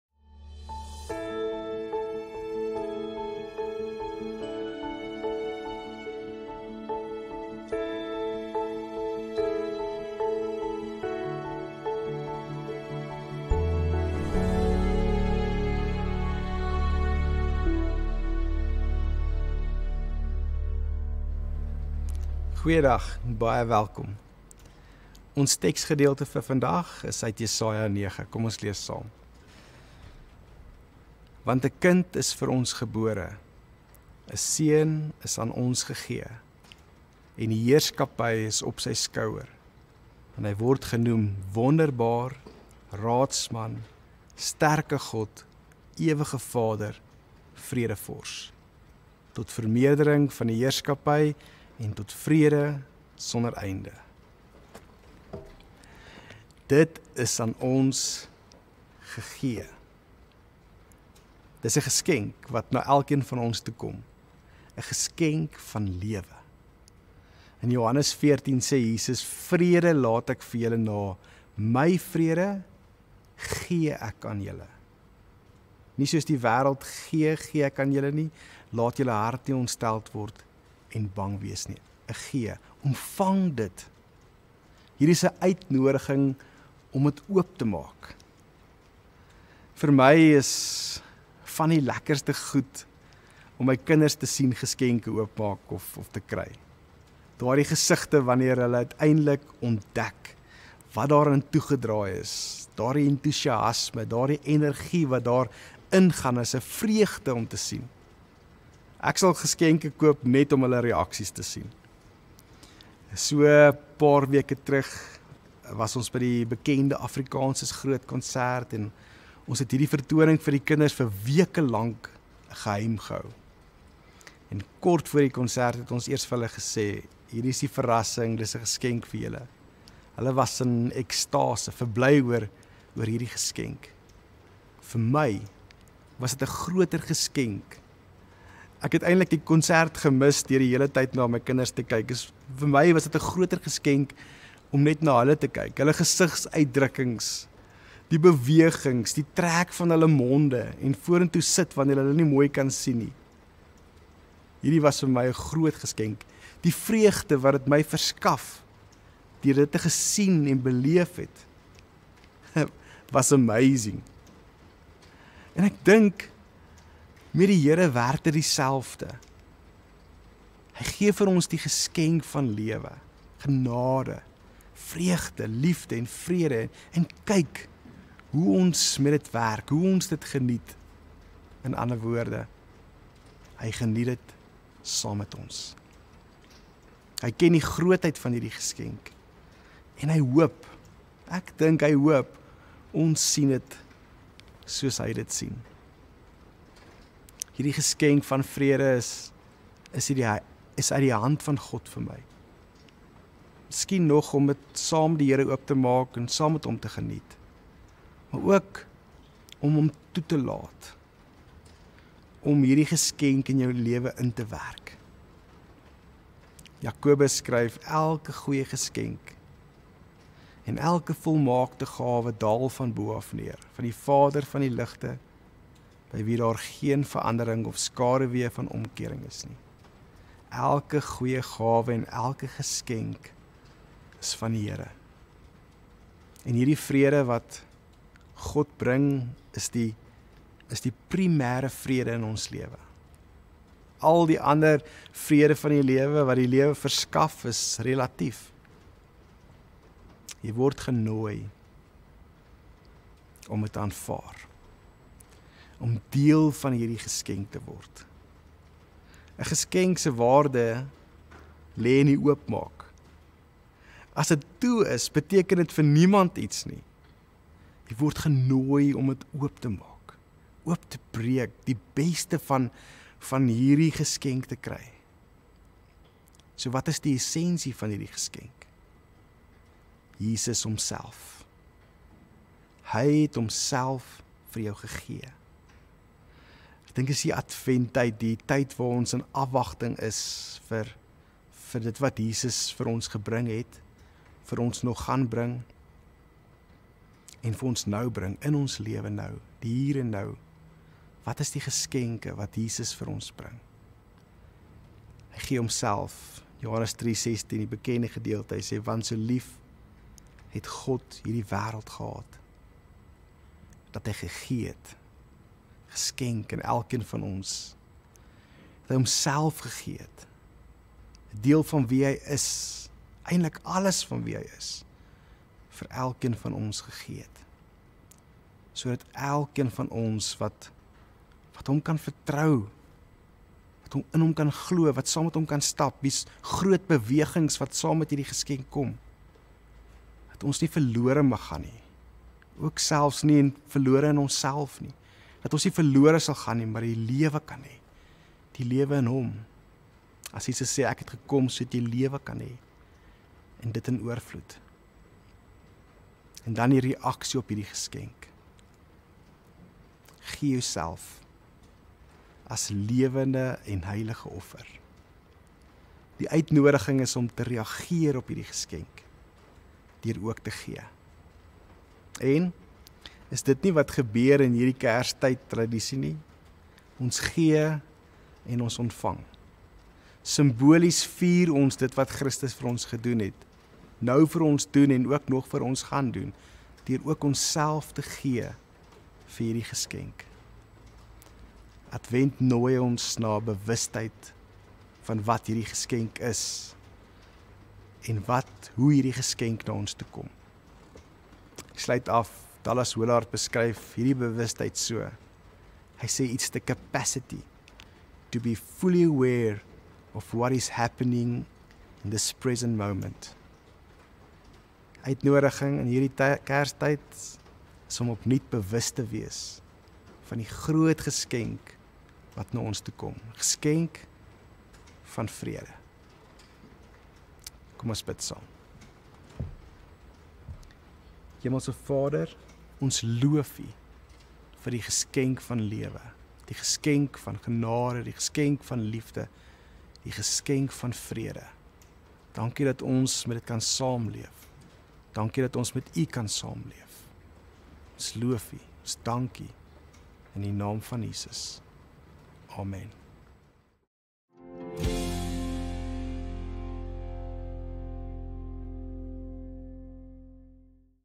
16 Desember 2022, Vrydag S6 Week 50 - Formasiereis
Preekreeks: 'n Tyd Met
Oordenkings: Vrede
Prediker: Melissa van Biljon